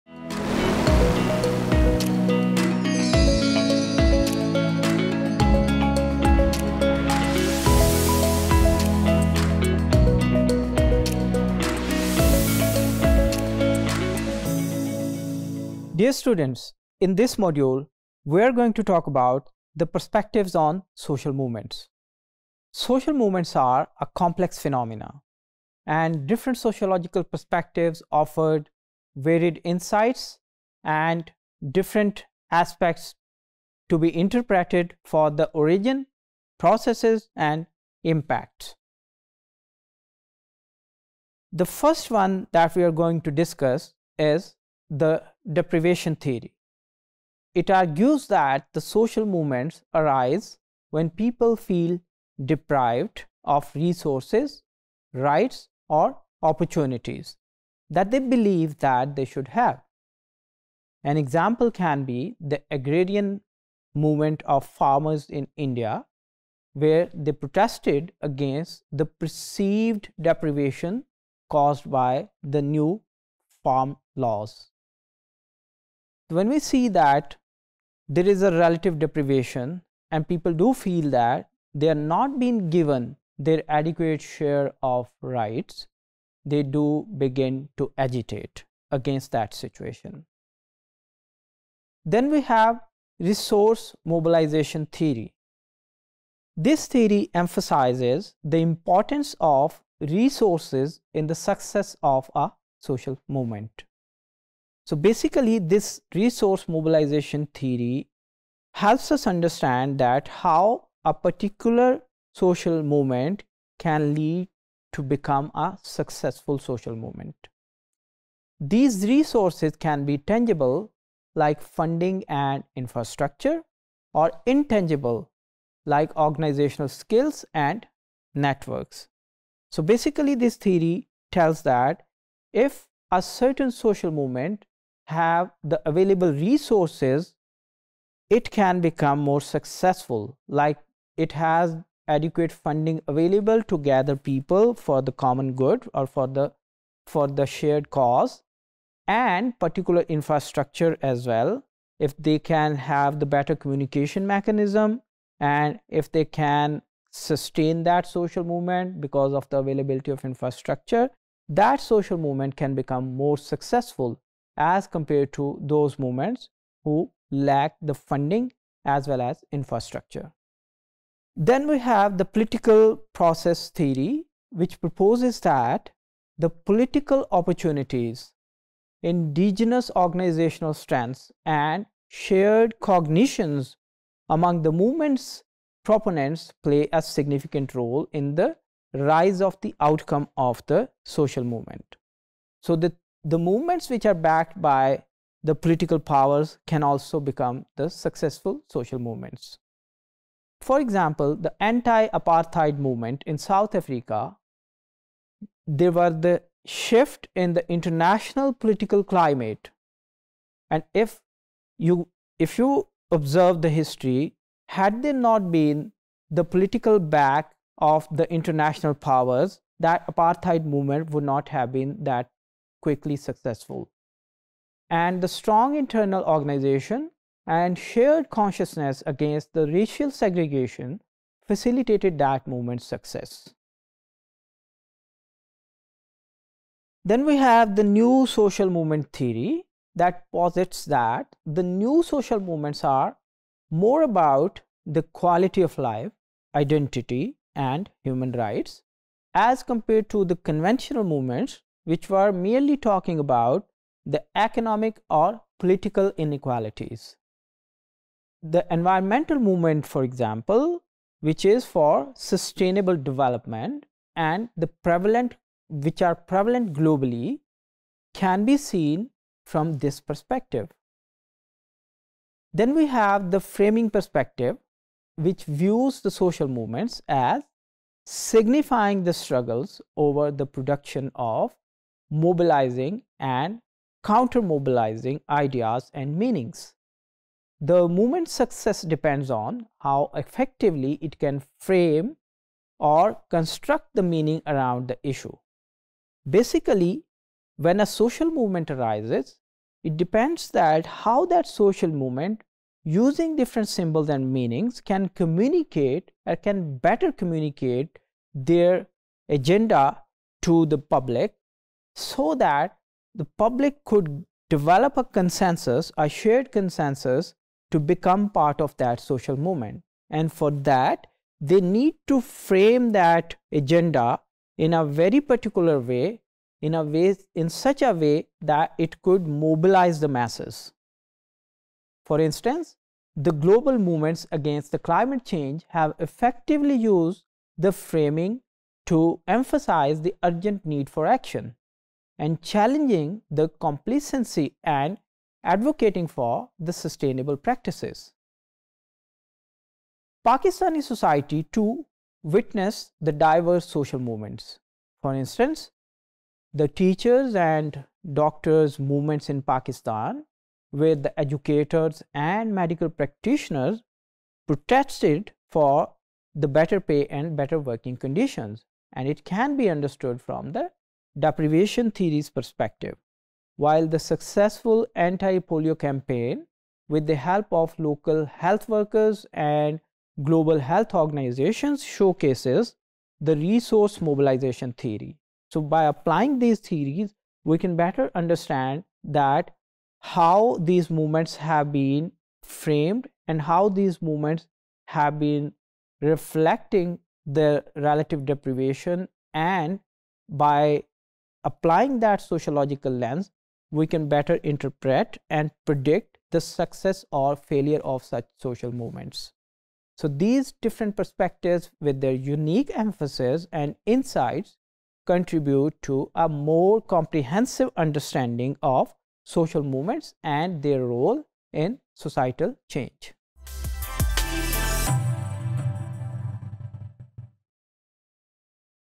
Perspectives on Social Movements | Introduction to Sociology | SOC101_Topic238
Topic238: Perspectives on Social Movements,
By Dr. Imran Sabir